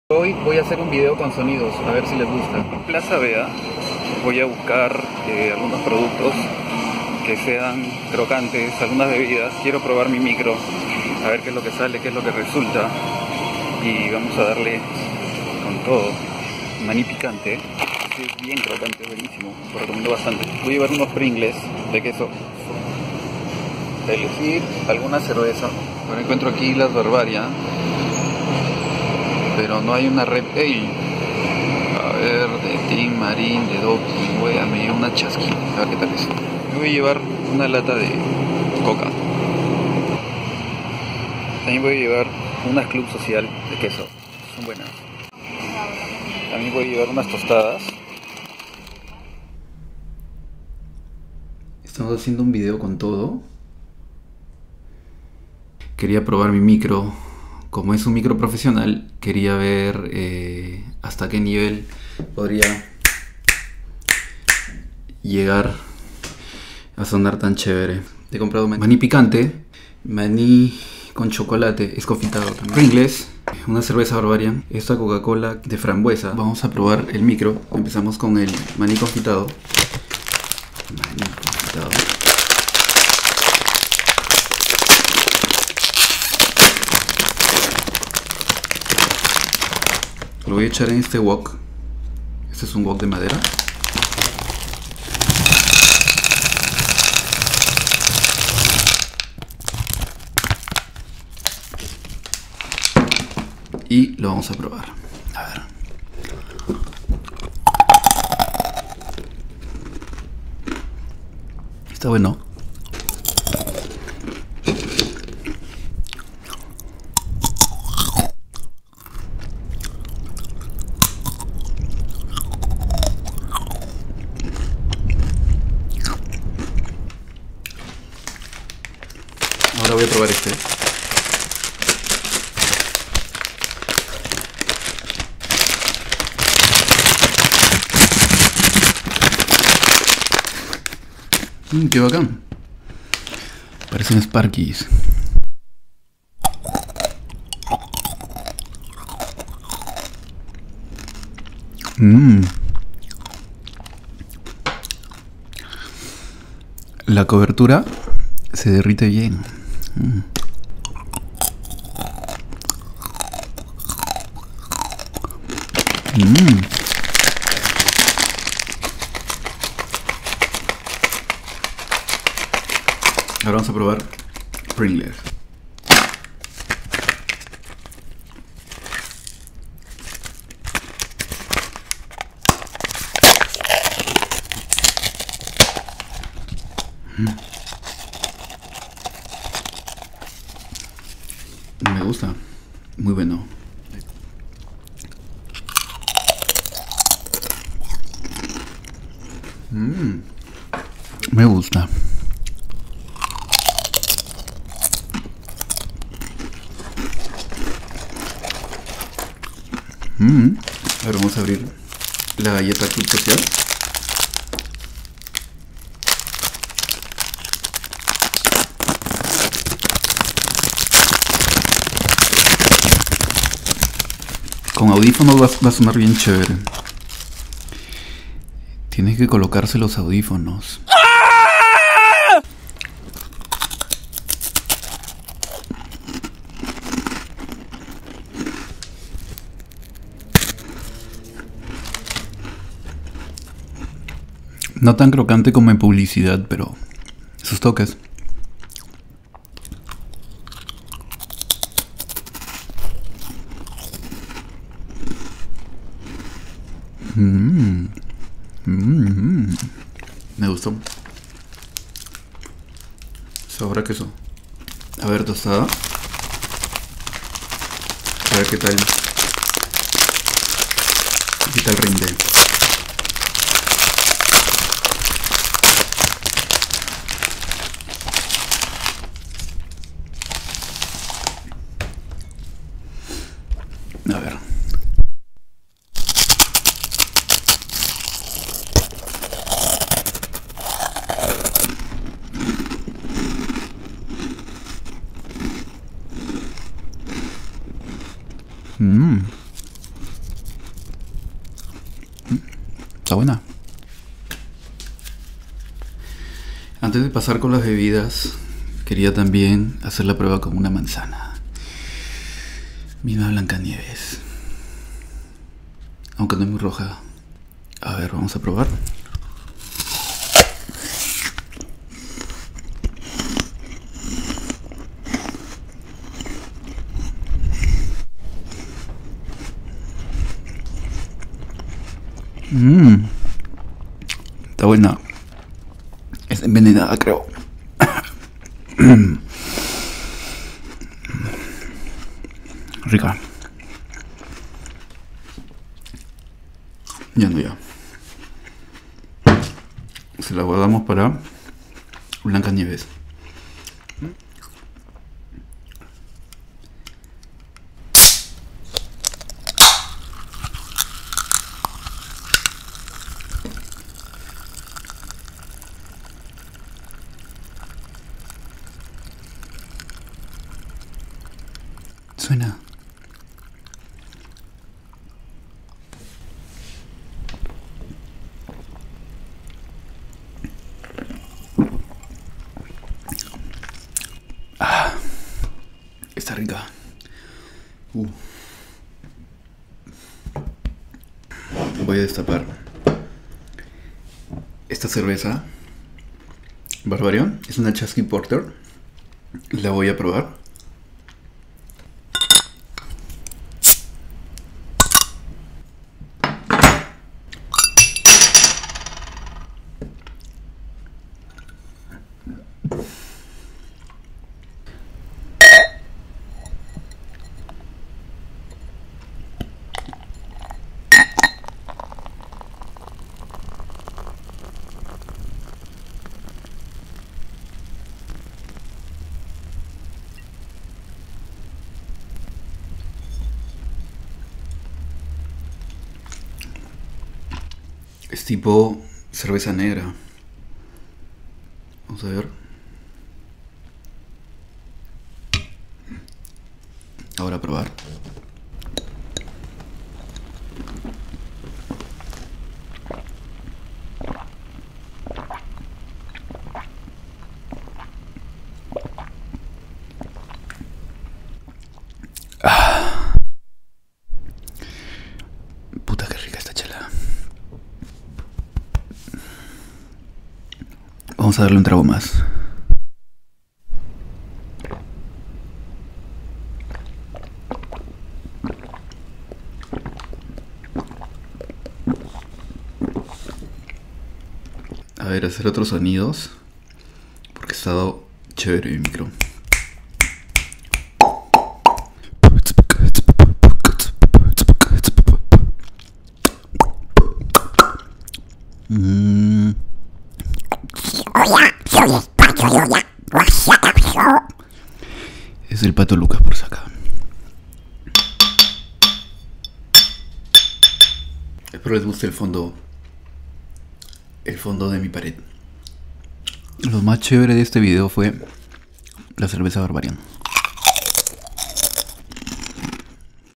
VLOG - FRANCO ANDRÉ - ASMR - PROBANDO SONIDOS
Amig@s en este video, quise probar mis micros profesionales, para mostrar la calidad de sonido que tienen, y a la vez jugar un poco con los sentidos del humano y las sensaciones que generan al escuchar todos los sonidos con audífonos. Espero y el video sea de su agrado, si desean que suba más videos ASMR con sonidos en particular, solo dejen un mensaje, y con gusto lo haré.

Espero lo disfruten, le den like al video, lo compartan y vean más de mis videos!! 😎👍🏻🙌🏻